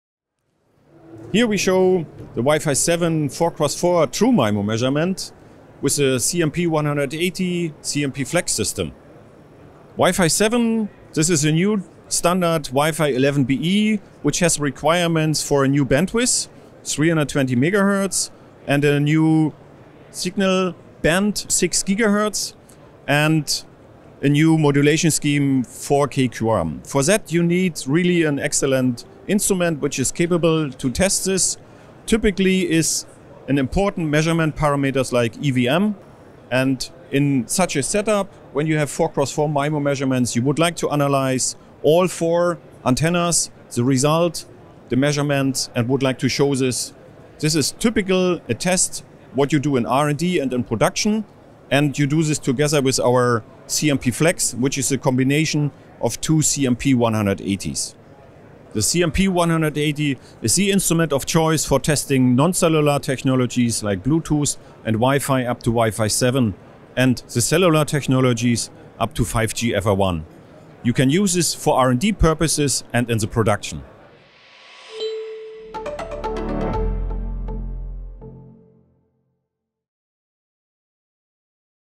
Wi-Fi 7 (IEEE 802.11be) 4x4 true-MIMO testing
Rohde & Schwarz demonstrates Wi-Fi 7 (IEEE 802.11be) 4x4 true MIMO measurements up to 320 MHz bandwidth and up to 6 GHz frequency band based on the R&S CMP180 CMPflexx system using a Wi-Fi 7 router as device under test (DUTs).
The R&S CMP180 radio communication tester is equipped with two analyzers, two generators and two times eight RF ports in a single box. By stacking several testers it can easily be scaled up to a CMPflexx system. The R&S CMP180 offers a frequency range up to 8 GHz and a bandwidth up to 500 MHz and enables simultaneous testing of multiple DUTs supporting cellular technologies up to 5G NR-FR1 and non-cellular technologies such as Bluetooth and Wi-Fi and many more.